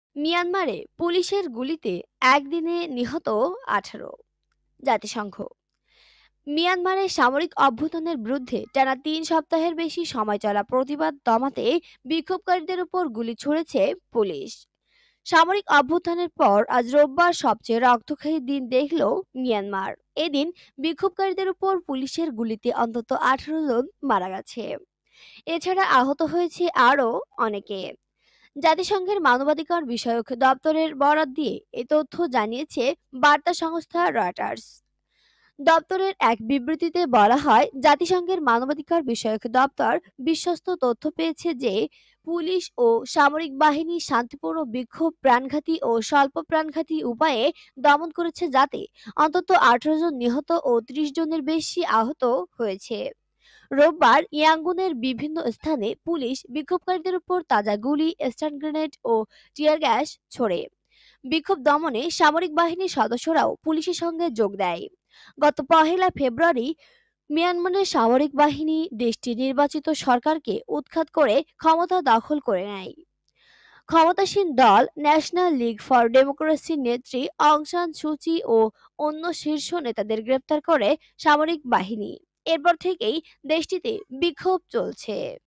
১৮ জনকে গুলি করে হ*ত্যা করলো পুলিশ | রক্তক্ষয়ী সংঘর্ষে মিয়ানমার উত্তাল | আন্তর্জাতিক সংবাদ বাংলা খবর
মিয়ানমারে পুলিশের গুলিতে একদিনে নিহত ১৮ : জাতিসংঘ
  মিয়ানমারে সামরিক অভ্যুত্থানের বিরুদ্ধে টানা তিন সপ্তাহের বেশি সময় চলা প্রতিবাদ দমাতে বিক্ষোভকারীদের উপর গুলি ছুড়েছে পুলিশ। সামরিক অভ্যুত্থানের পর আজ রোববার সবচেয়ে রক্তক্ষয়ী দিন দেখল মিয়ানমার। এদিন বিক্ষোভকারীদের উপর পুলিশের গুলিতে অন্তত ১৮ জন মারা গেছে। এ ছাড়া আহত হয়েছে আরও অনেকে। জাতিসংঘের মানবাধিকারবিষয়ক দপ্তরের বরাত দিয়ে এ তথ্য জানিয়েছে বার্তা সংস্থা রয়টার্স। দপ্তরের এক বিবৃতিতে বলা হয়, ‘জাতিসংঘের মানবাধিকার বিষয়ক দপ্তর বিশ্বস্ত তথ্য পেয়েছে যে, পুলিশ ও সামরিক বাহিনী শান্তিপূর্ণ বিক্ষোভ প্রাণঘাতী ও স্বল্প প্রাণঘাতী উপায়ে দমন করেছে যাতে অন্তত ১৮ জন নিহত ও ৩০ জনের বেশি আহত হয়েছে।’ রোববার ইয়াঙ্গুনের বিভিন্ন স্থানে পুলিশ বিক্ষোভকারীদের ওপর তাজা গুলি, স্টান গ্রেনেড ও টিয়ার গ্যাস ছোড়ে। বিক্ষোভ দমনে সামরিক বাহিনীর সদস্যরাও পুলিশের সঙ্গে যোগ দেয়। গত ১ ফেব্রুয়ারি মিয়ানমারের সামরিক বাহিনী দেশটির নির্বাচিত সরকারকে উৎখাত করে ক্ষমতা দখল করে নেয়। ক্ষমতাসীন দল ন্যাশনাল লিগ ফর ডেমোক্র্যাসির নেত্রী অং সান সু চি ও অন্য শীর্ষ নেতাদের গ্রেপ্তার করে সামরিক বাহিনী। এর পর থেকেই দেশটিতে বিক্ষোভ চলছে।